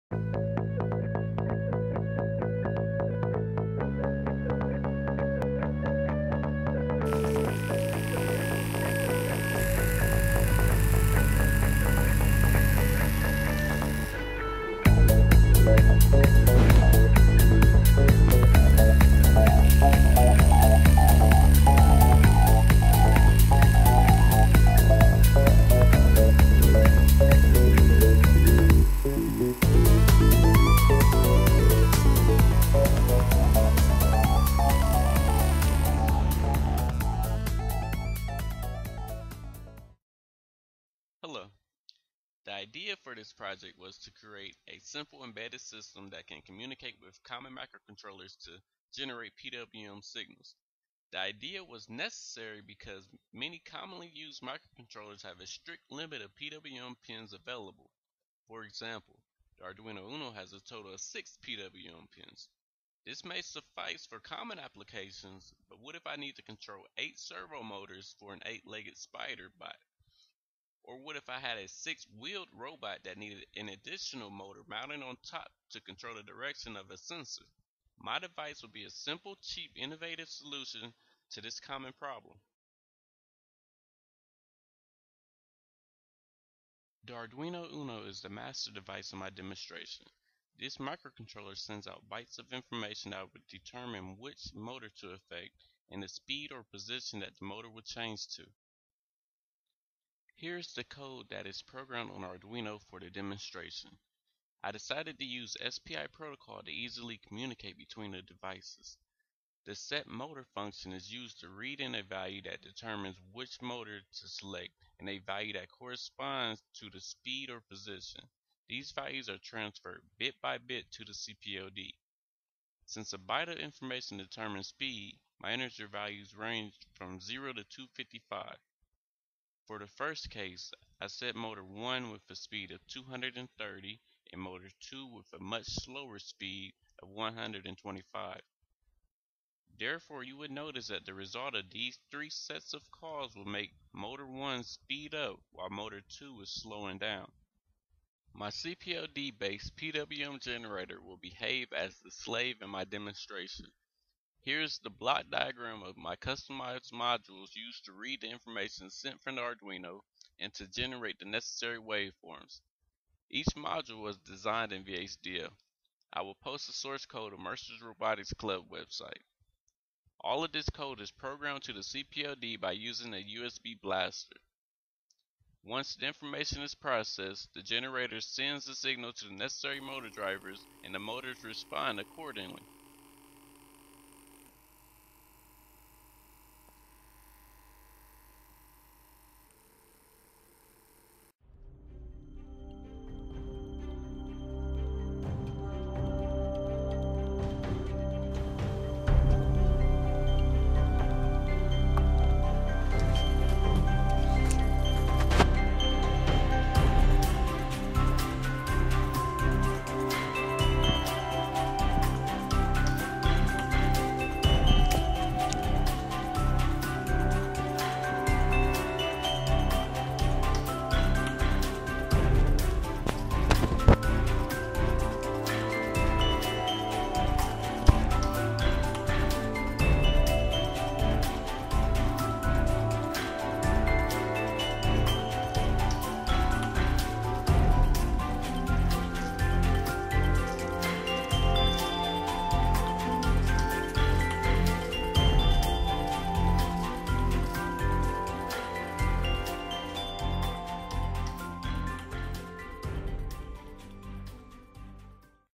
CPLD PWM Generator
This video is of my CPLD PWM Generator Project. I used an Altera 3000A 64 Macrocell CPLD. More information can be found on Mercer University's Robotics Club website.